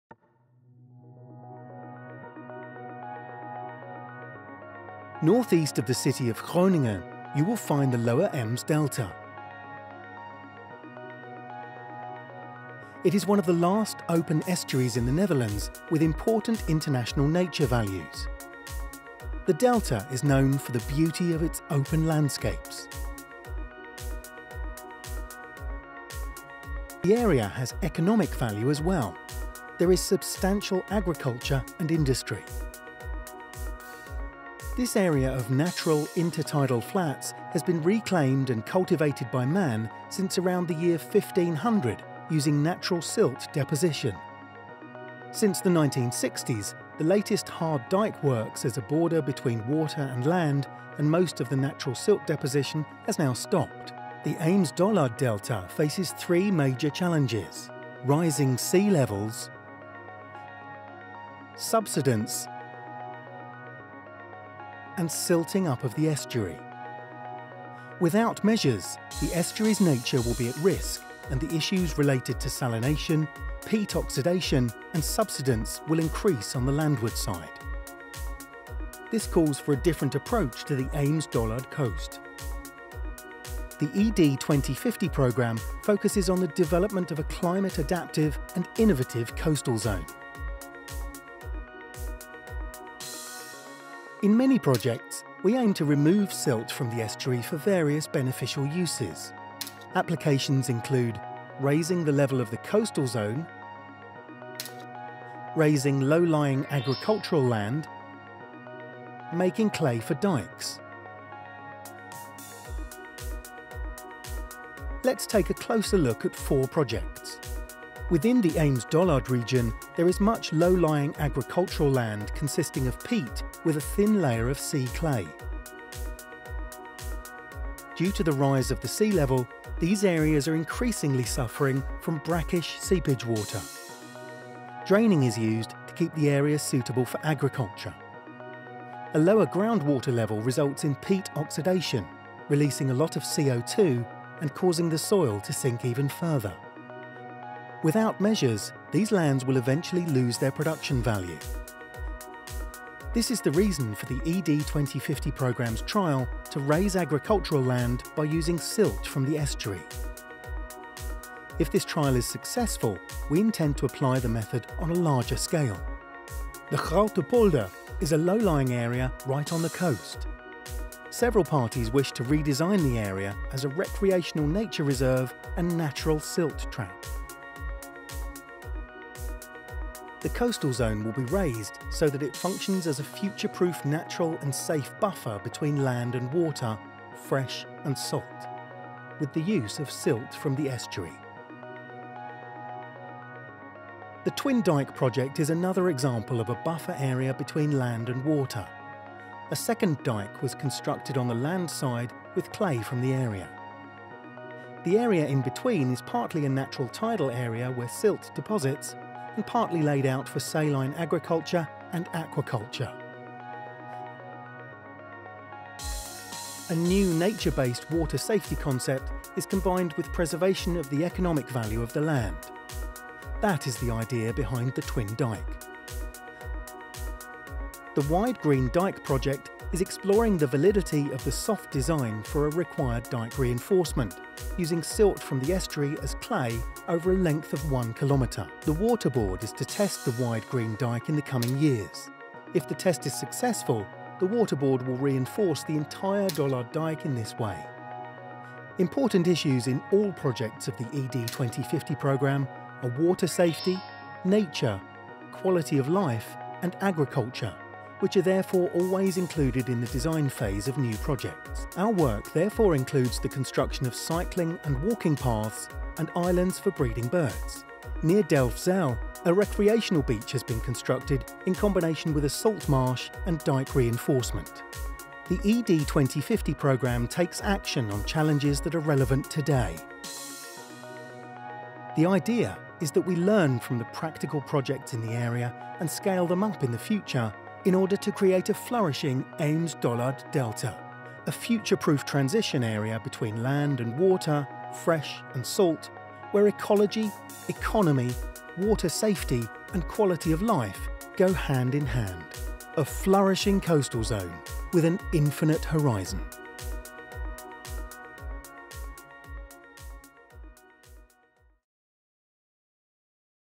Climate Adaptation in the Ems-Dollard delta (English)
This film shows how we adapt the Ems-Dollard coast in theNorthern Netherlands to the effects of climate change, such as salinization, peat oxidation and soil decline. Without measures, nature is in danger of being lost in this unique area and the nuisance behind the sea dyke is increasing. We want to avoid this with various projects under the Ems-Dollard 2050 program, such as the Twin Dyke, the raising of agricultural land with sludge, the Broad Green Dyke and Groote Polder.